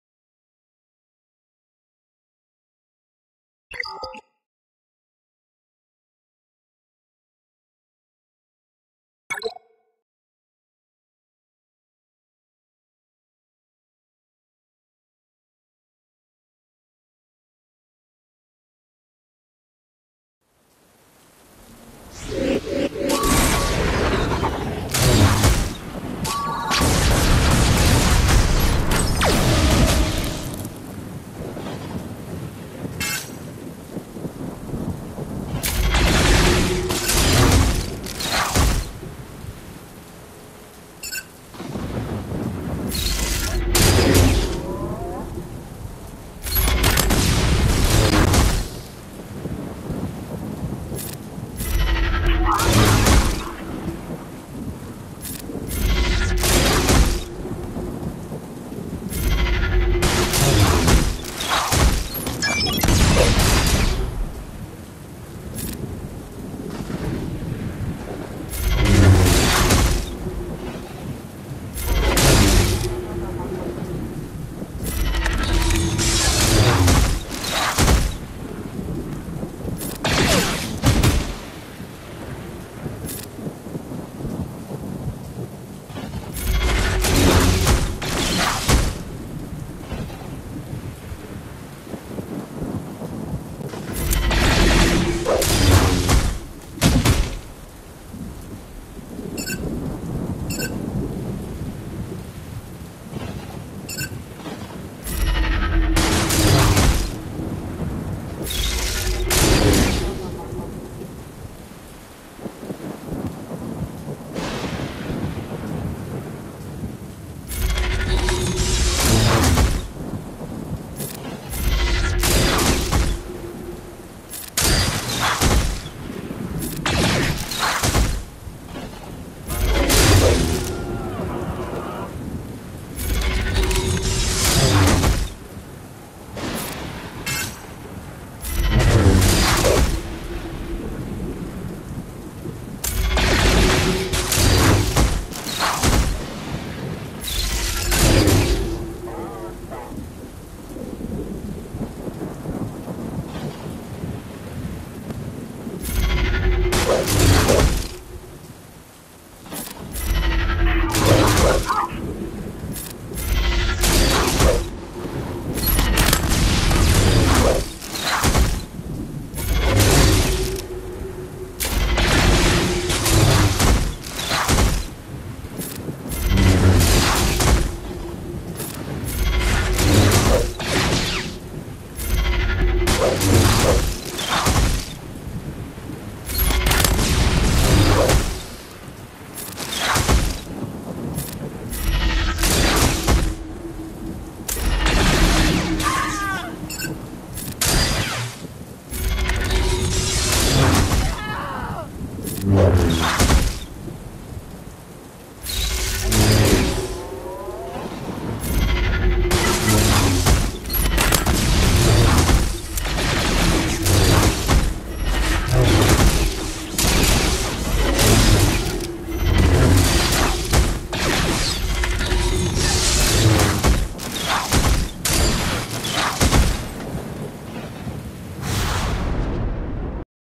Hera/Sabine/Kanan/Chopper/Rex vs. Admiral Raddus/Jyn/Cassian/SRP/K-2SO (15% HP): 65 banners
Given the tenacity on K-2, I should have used Choppers buff dispel on him right away.